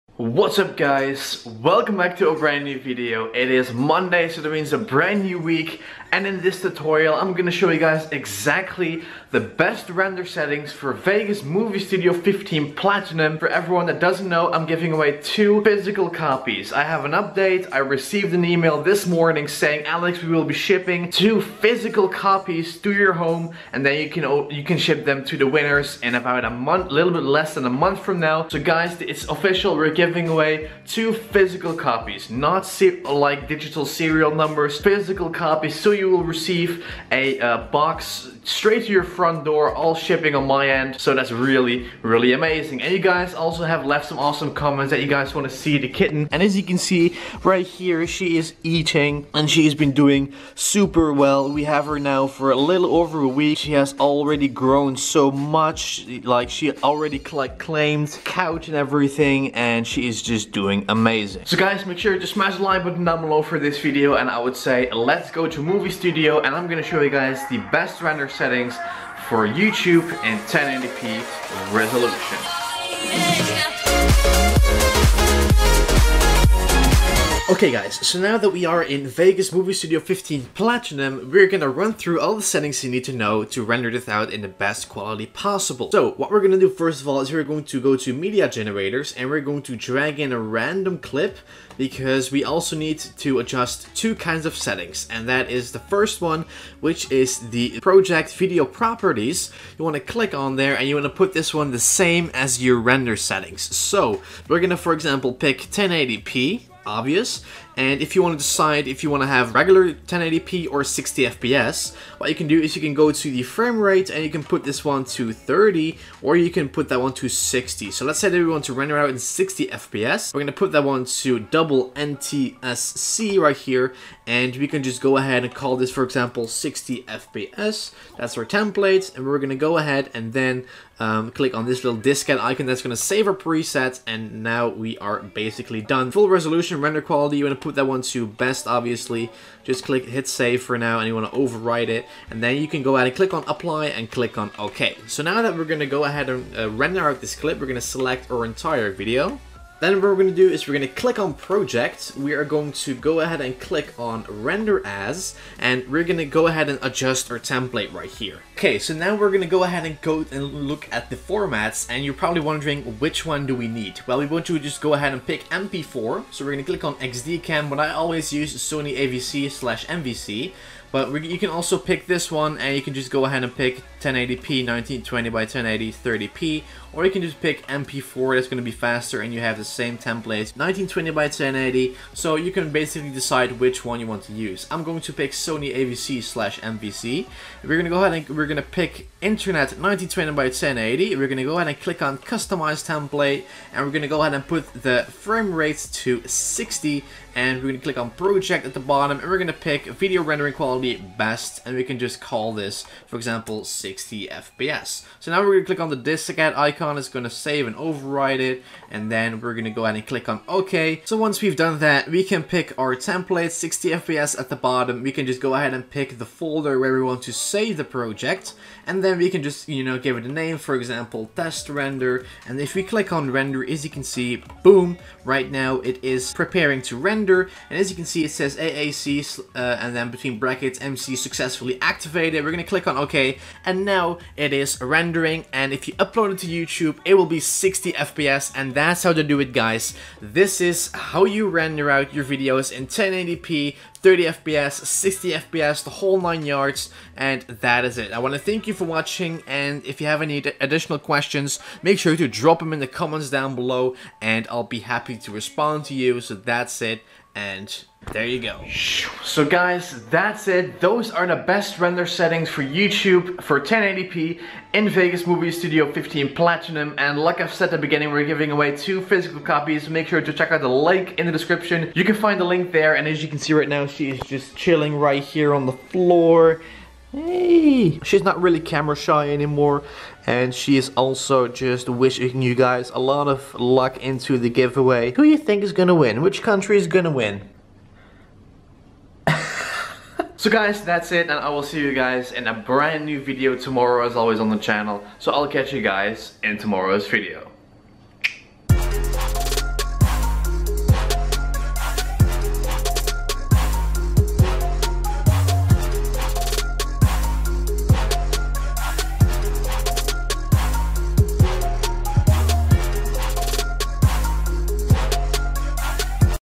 VEGAS Movie Studio 15: Best Render Settings For YouTube (1080p)
- - Welcome back everyone! Today I'm going to show you the best 1080p render settings for YouTube! Make sure to SMASH that like button below as well! Stick around for more content!

Pick up VEGAS Movie Studio here!